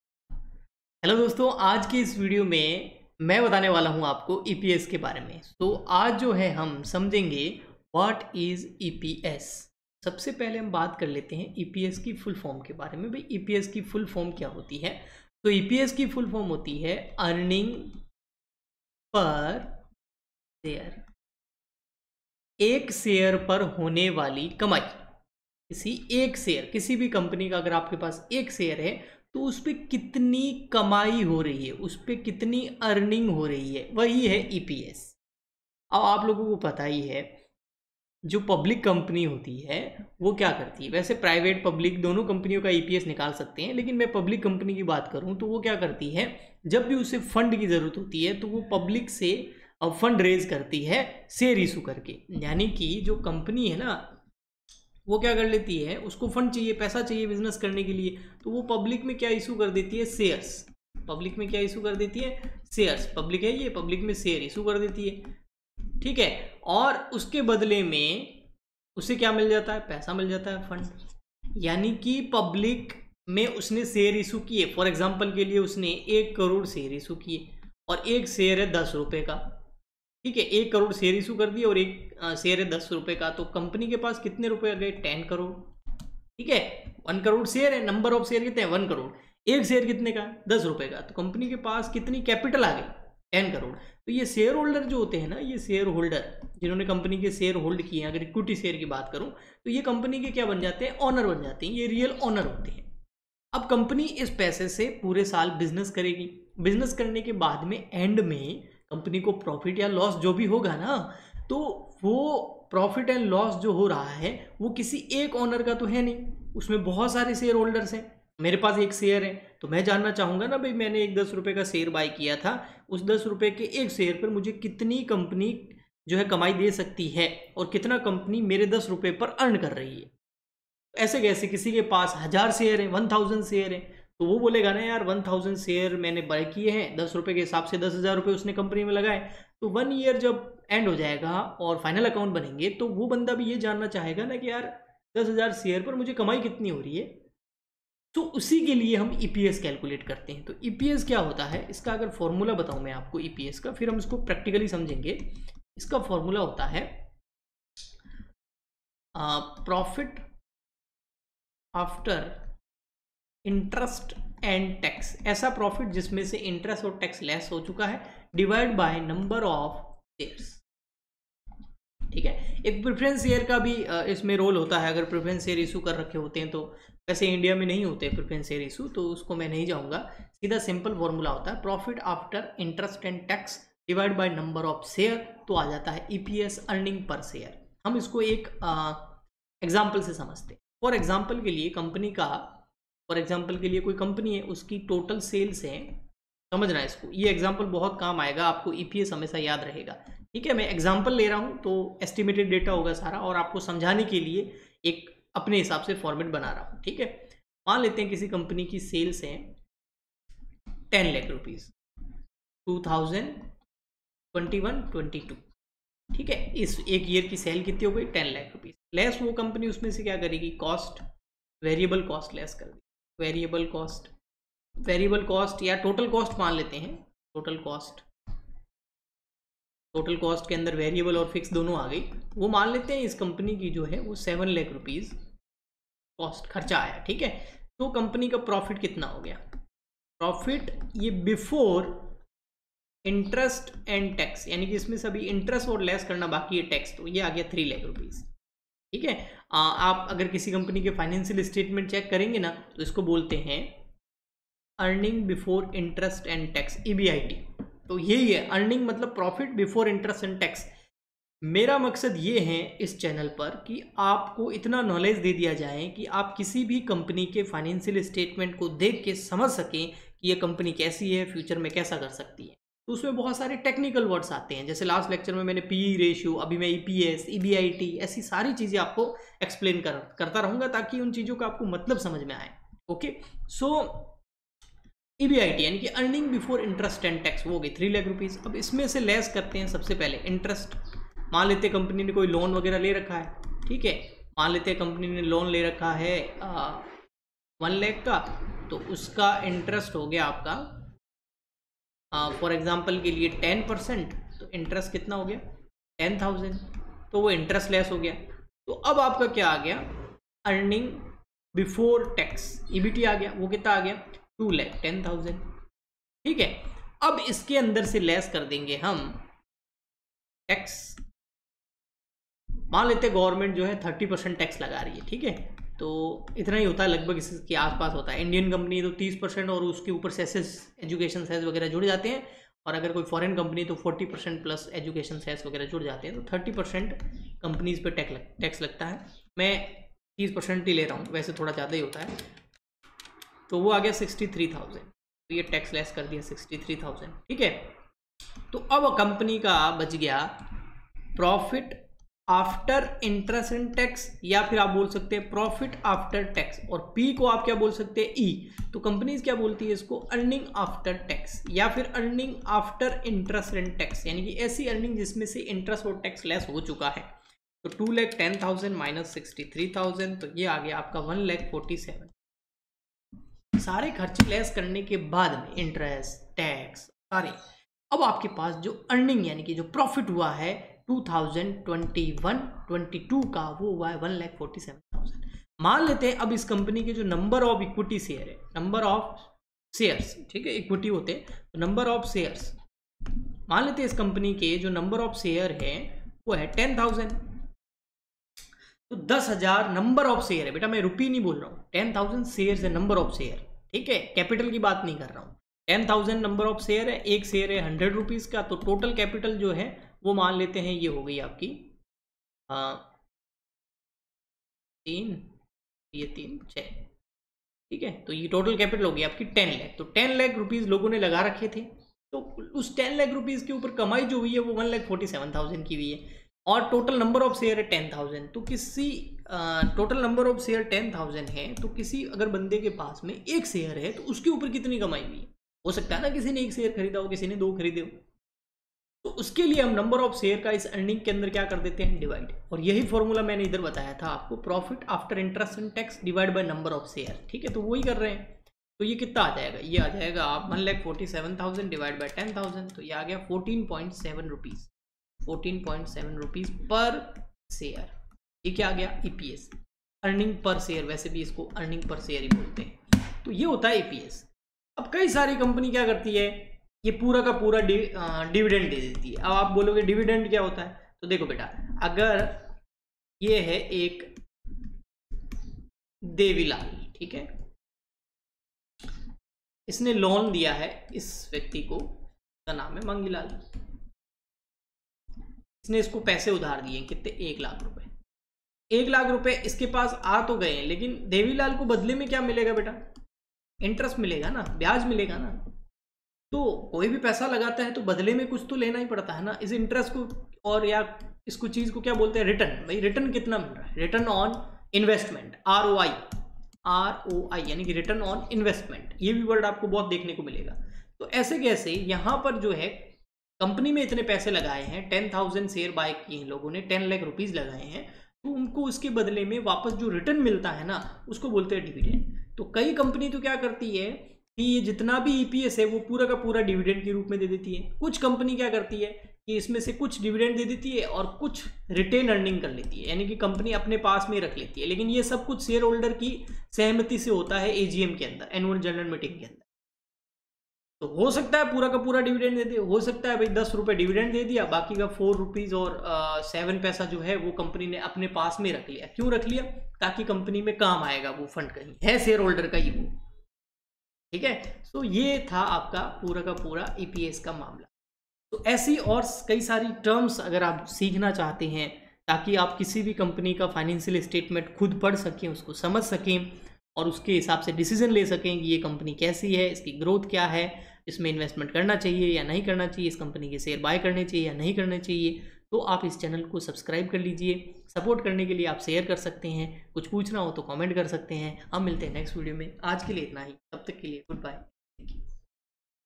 What is EPS ? | EPS in stock market | Earning Per Share | stock market for beginners
In this video i was try to expalin What is EPS in stock Marekt. Earning Per share understading is most important for your financial education. Its help you for fundamental analysis of any stock.